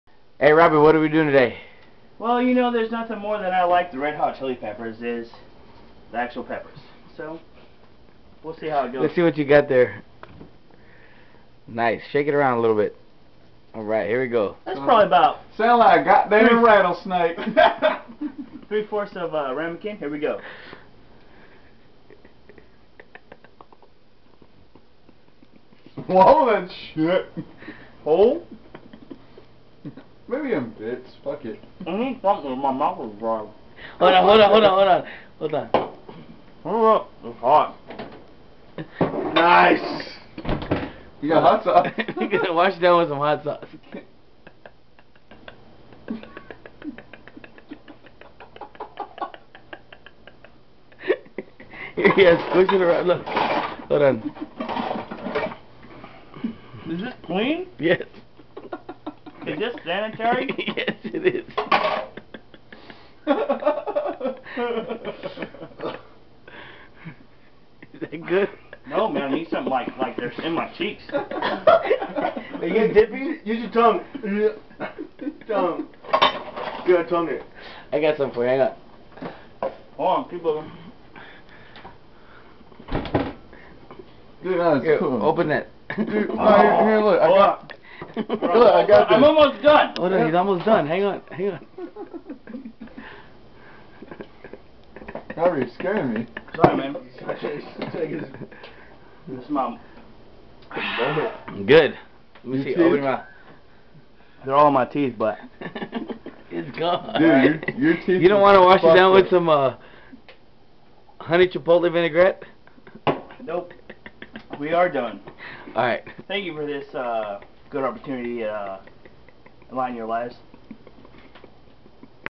Robby Does: Chili Peppers
Robby takes a shot of chili peppers and washes it down with hot sauce!! HOTTTTTT!